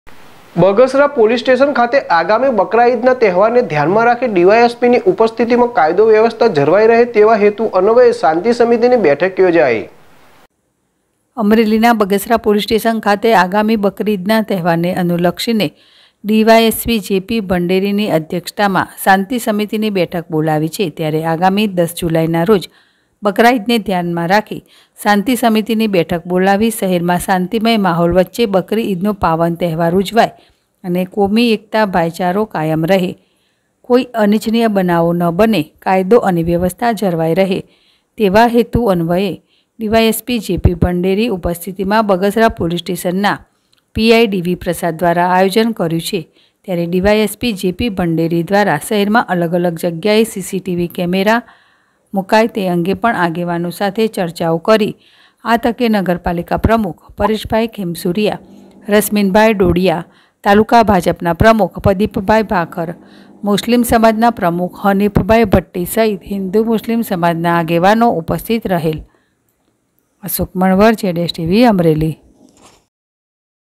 બગસરા પોલીસ સ્ટેશન ખાતે આગામી બકરી ઇદના તહેવારને ધ્યાને રાખીને DYSP ની ઉપસ્થિતિ માં કાયદો વ્ય
બગસરા પોલીસ સ્ટેશન ખાતે આગામી બકરી ઇદના તહેવારને ધ્યાને રાખીને DYSP ની ઉપસ્થિતિ માં કાયદો વ્યવસ્થા જળવાઈ રહે તેવા હેતુ અન્વયે શાંતિ સમિતિની બેઠક યોજાઇ